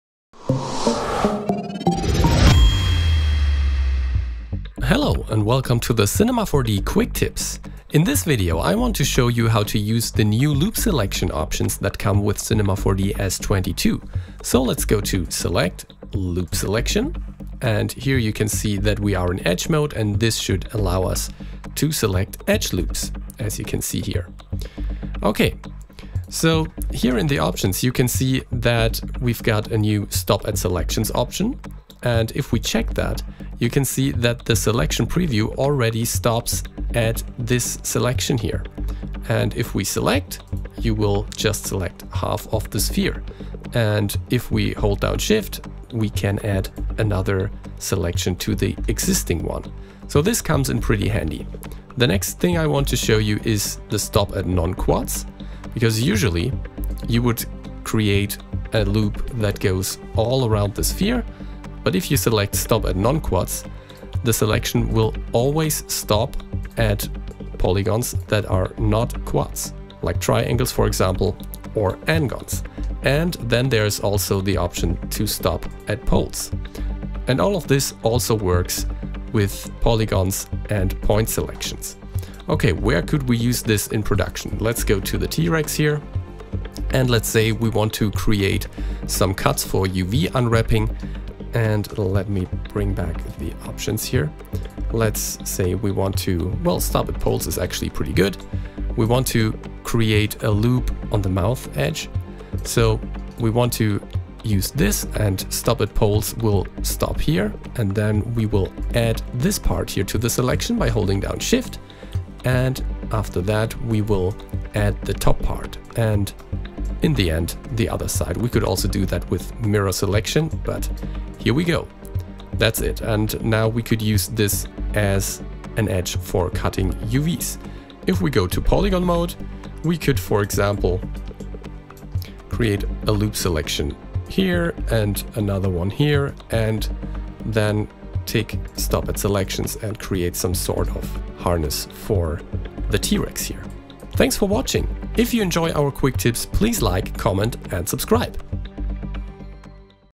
Tip - 294: How to use the new Loop Selection options in Cinema 4D S22
In this Quick Tip, Jonas Pilz shows you how to use the Loop Selection tool in Cinema 4D S22. You will quickly notice how useful the new options Stop at Selection, Stop at Non-Quad and Stop at Poles are in certain situations.

Recorded in Cinema 4D S22.